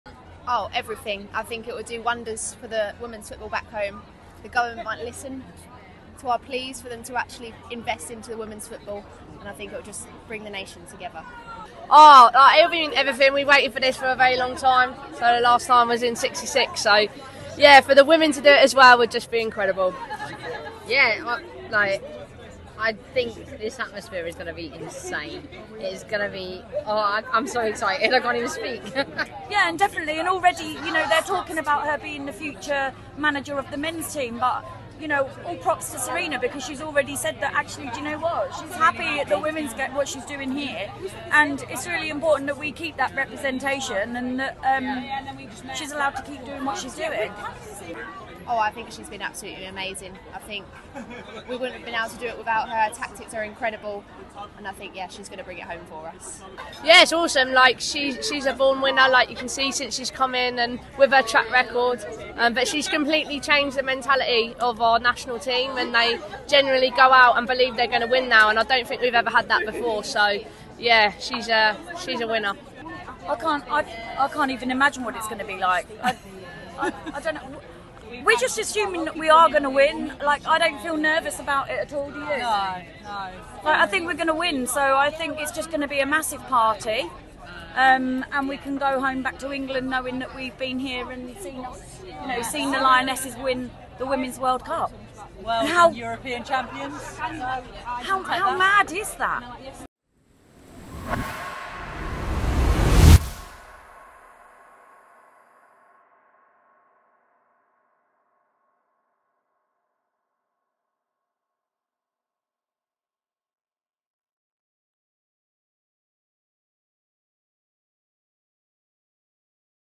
"IT WILL MEAN EVERYTHING!" England fans in Australia are preparing for the World Cup final v Spain
The Lionesses face Spain in the Women's World Cup final.

England are hoping to win their first ever Women's World Cup in Australia.

Sarina Wiegman's side are the current European champions.